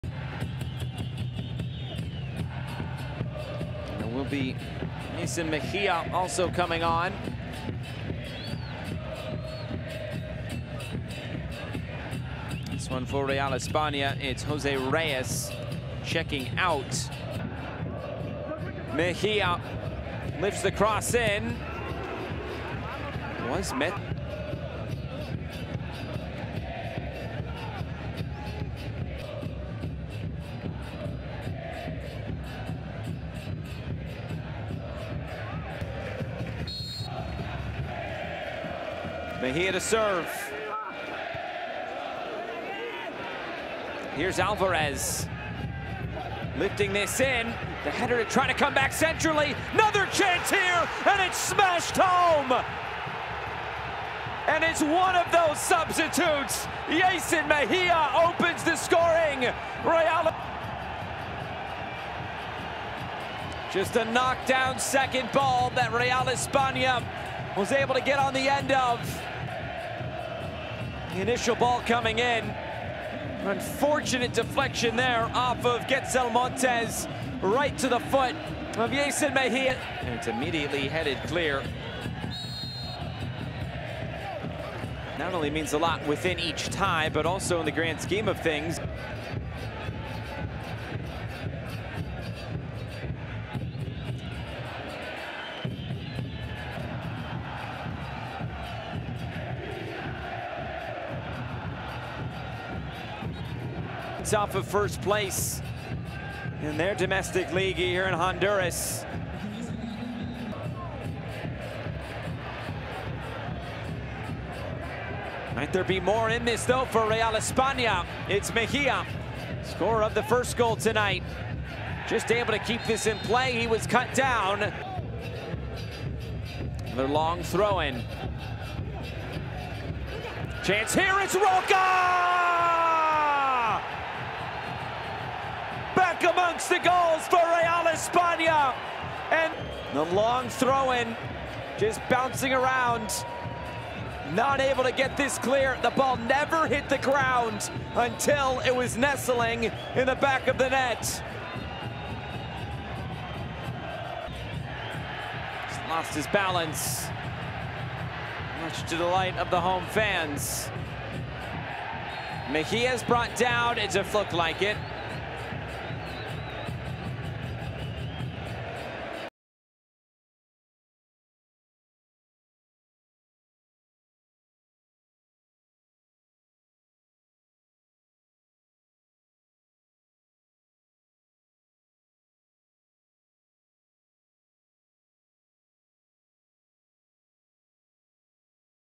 Yeison Mejía vs Herediano | Concacaf League Cuartos de Final 2022
Yeison Mejía vs Herediano

Publicaré videos sobre el fútbol de Honduras. Ayúdame en mi viaje hacia 5000 suscriptores, gracias por todo.